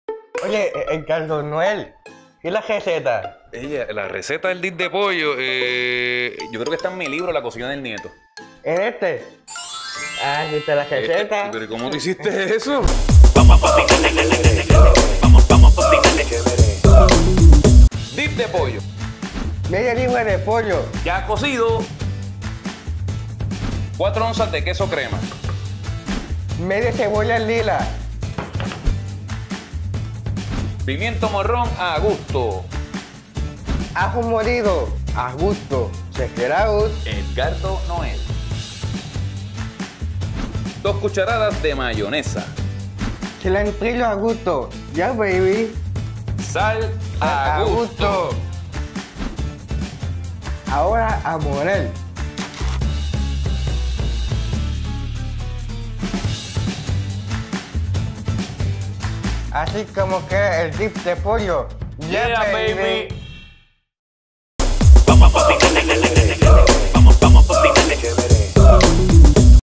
Cómo Hacer Dip de Pollo
Junto a mi amigo el Chef Chris, te enseñaremos a preparar la receta de Dip de pollo que tanto necesitabas, fácil, rápida y deliciosa.

 Para más recetas como esta 👇🏼👇🏼👇🏼